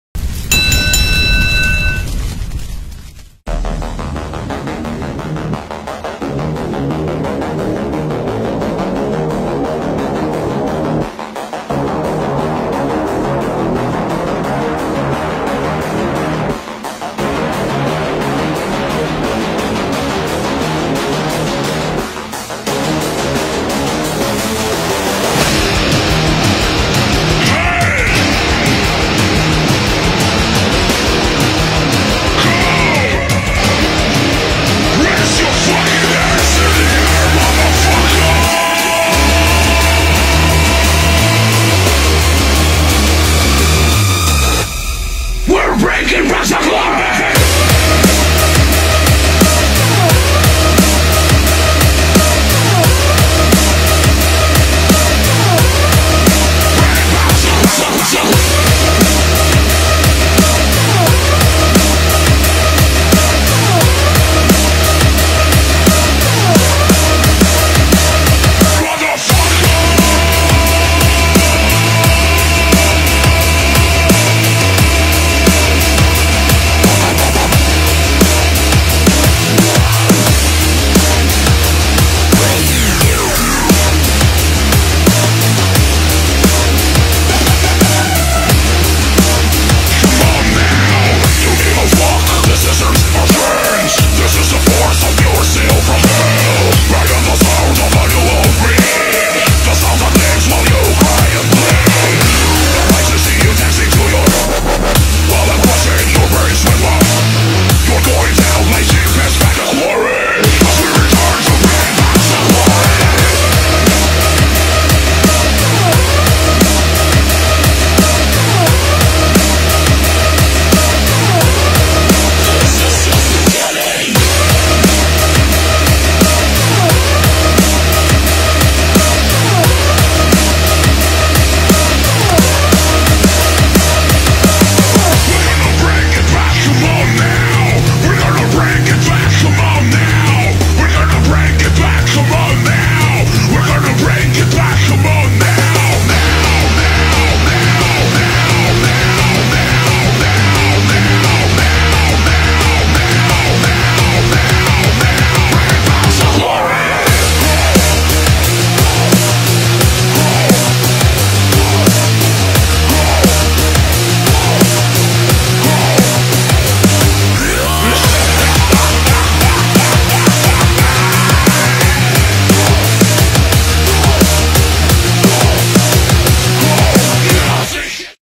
MFC 10 | Hassan Mandour (EGP) VS. Gadzhimurad Gadzhimuradov (RUS)
Monster Fighting Championship
MFC 10
Cairo, Egypt
25th Of August 2022
Bantamweight Bout 135 LBS
Hassan Mandour VS. Gadzhimurad Gadzhimuradov
Hassan Mandour won by TKO in 1:33 of the second Round